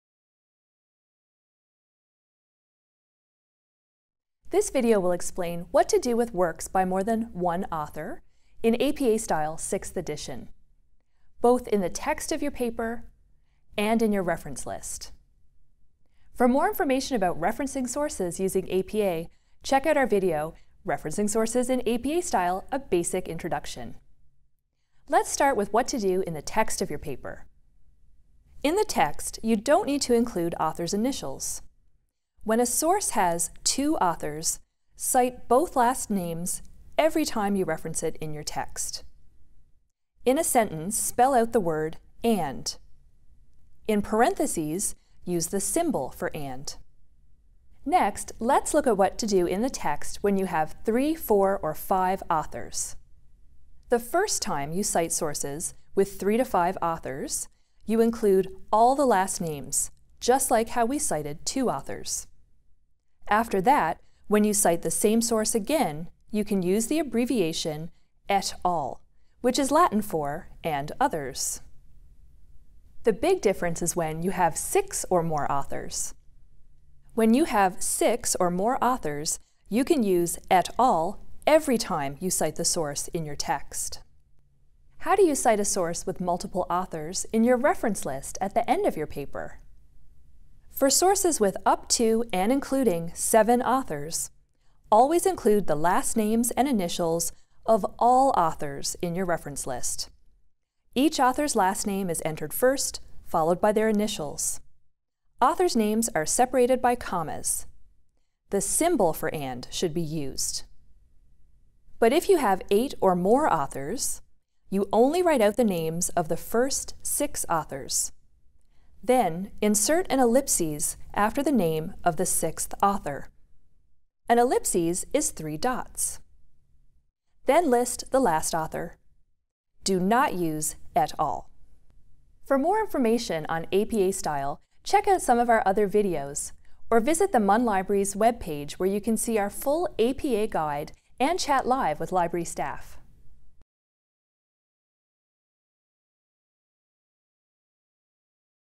How to Reference Multiple Authors in APA Style
This video is based on the 6th edition of the Publication Manual of the American Psychological Association. It explains what do with works by more than one author in the text of your paper and in your Reference List and when to use "et al.".
In-text:
2 authors (0:28)
3-5 authors (0:47)
6 or more authors (1:12)
Reference List:
2-7 authors (1:30)
8 or more authors (1:51)